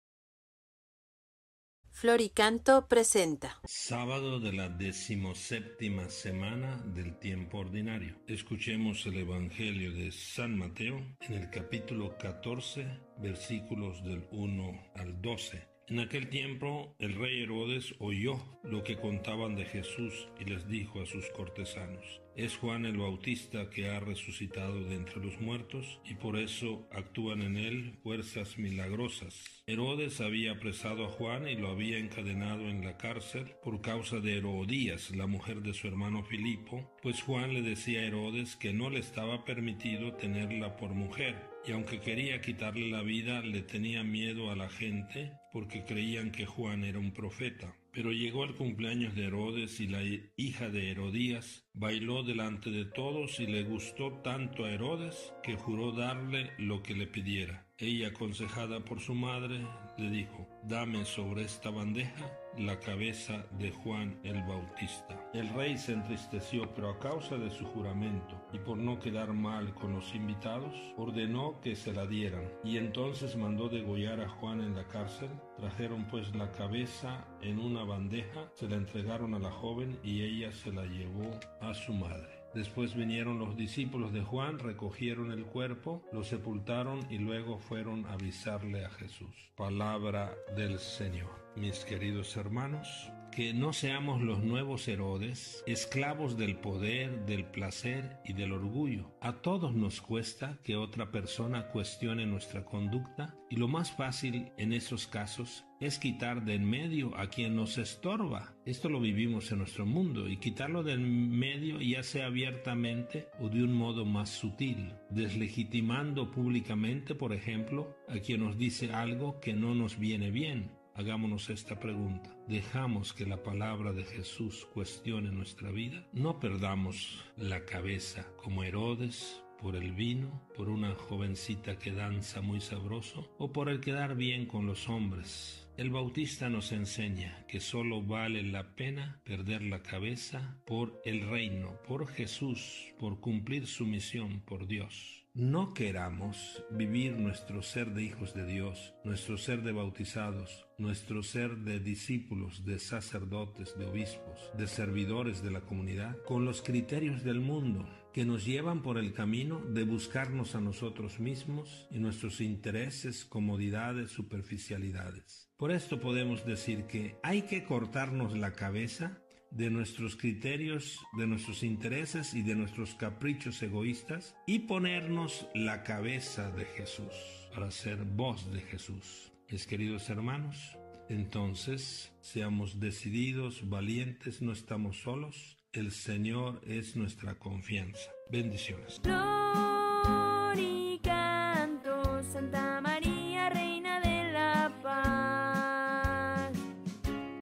XVII Sábado de Tiempo Ordinario. 3 de Agosto del 2024.
Como un homenaje póstumo y para que sus mensajes tan ricos y llenos De Dios sigan evangelizando hicimos esta producción con mucho respeto y cariño, sabiendo que a Mons. Fabio le gustaría seguir llevando a Cristo a muchas almas.